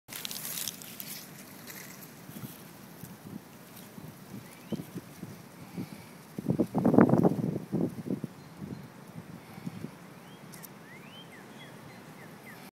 Midland Painted Turtle
A male midland painted turtle sliding into the water.